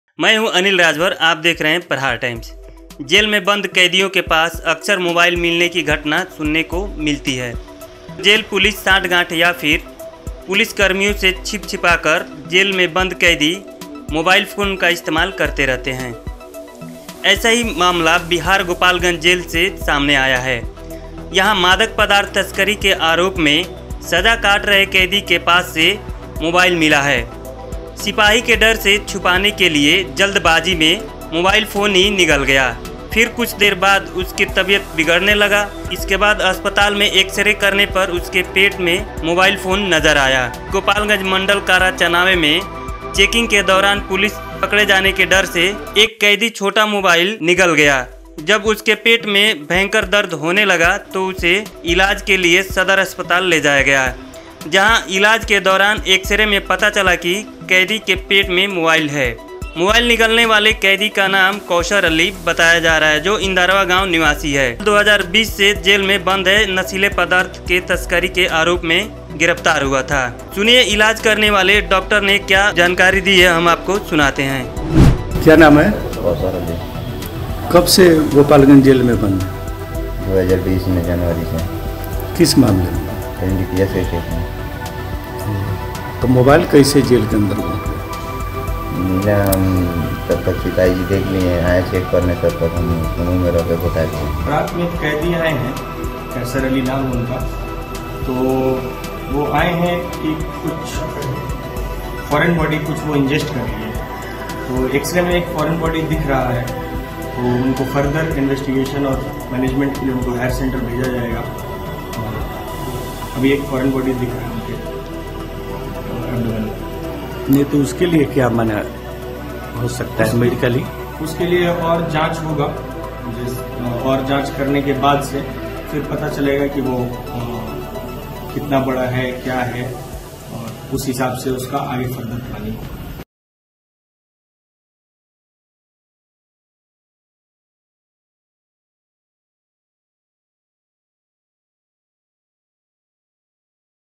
पुलिस की डर से कैदी ने निगला मोबाइल || Anil Rajbhar || Aapka Prahar Times
पुलिस की डर से कैदी ने निगला मोबाइल || Anil Rajbhar ||